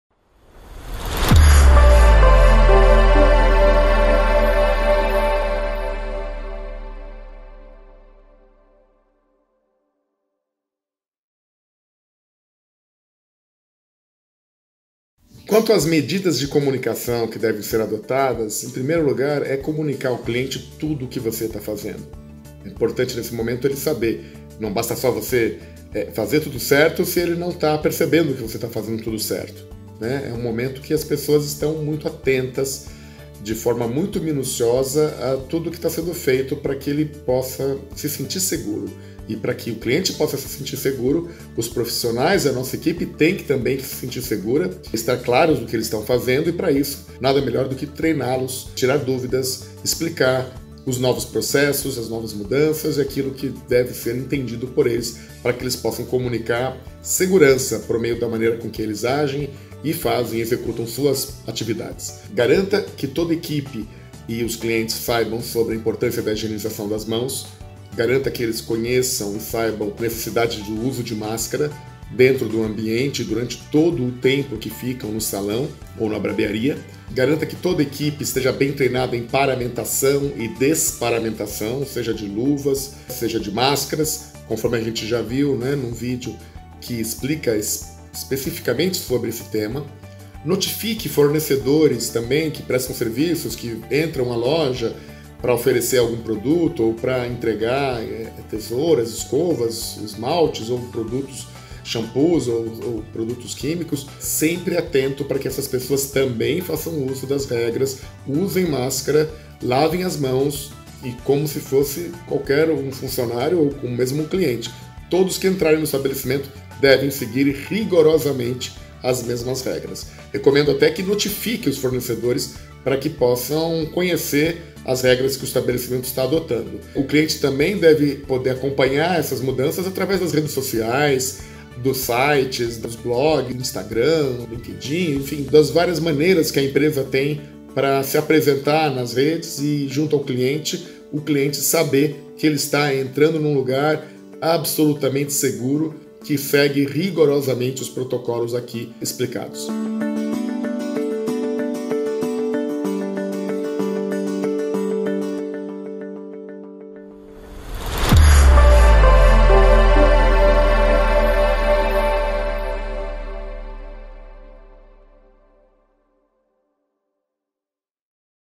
Vídeo 4 – Protocolos Covid para Salões de Beleza e Barbearia: Comunicação
Comunicação Assertiva para Colaboradores, Prestadores e Clientes sobre os protocolos adotados

Créditos:
Realização: Senac
Produção: Canal 7 Vídeo Design
Desenvolvimento e nararção: prof. Marcelo Boeger